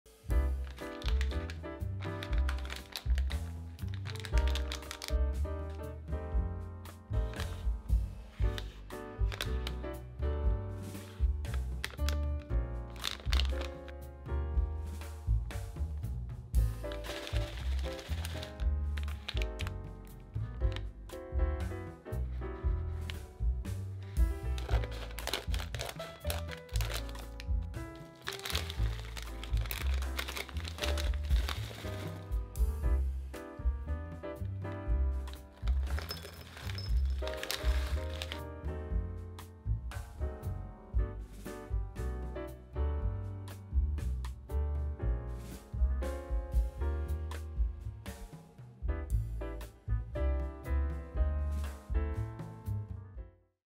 MINI OREO POPCORN
Opening MINI OREO COOKIE POPCORN.